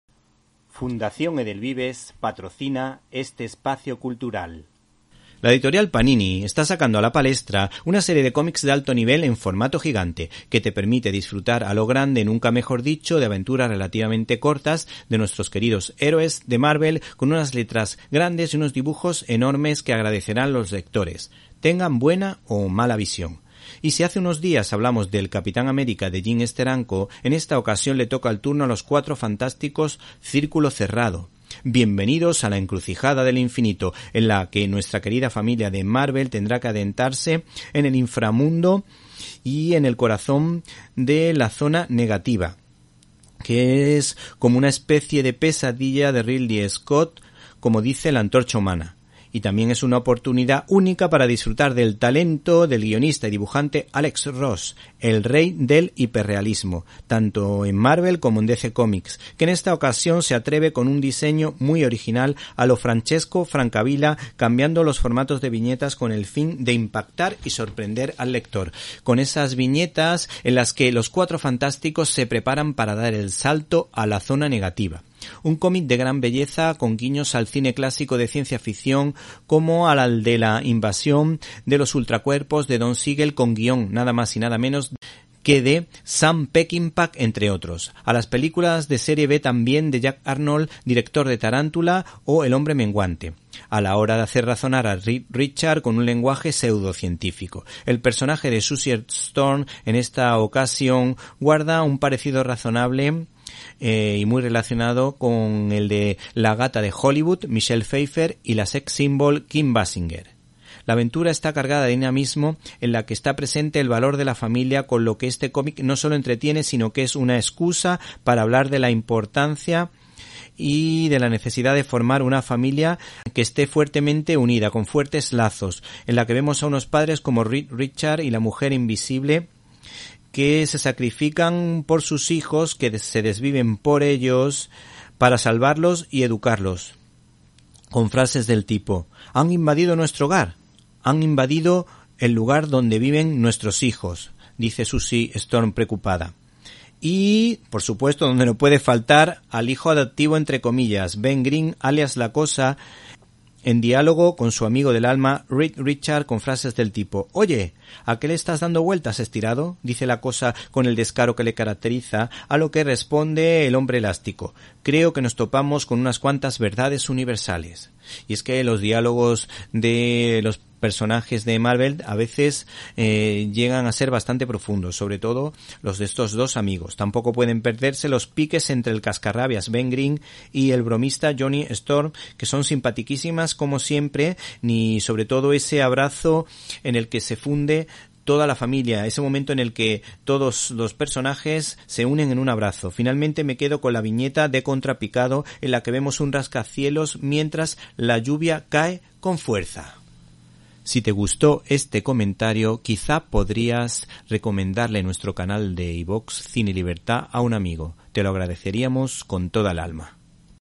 Los 4 Fantásticos: Círculo cerrado-El hiperrealismo y el sacrificio por los hijos
Los 4 Fantásticos: Círculo cerrado-El hiperrealismo y el sacrificio por los hijos. ¿Cuál es el verdadero significado de la amistad?¿Qué serías capaz de hacer por tus hijos?

Aquí puedes escuchar mi comentario...

La editoria Panini nos habla de su cómic: Los 4 Fantásticos: Círculo cerrado-El hiperrealismo y el sacrificio por los hijos

Después de que un intruso aparezca en el Edificio Baxter, Los Cuatro Fantásticos se encuentran rodeadores de un enjambre de parásitos. Proceden de la Zona Negativa y han llegado a la Tierra a través de un huésped humano. ¿Con qué propósito? ¿Quién está detrás de ellos? La Primera Familia debe acudir a la Zona Negativa para averiguarlo. Marvel se enorgullece en presentar la primera obra de una nueva colección de gran formato, en colaboración con Abrams ComicArts, con el aclamado Alex Ross revisitando una historia clásica de Stan Lee y Jack Kirby. ¡Nada es imposible y todo puede pasar!

Te ofrecemos una selección de tebeos para que puedas conocer más detalles de esta familia de superhéroes...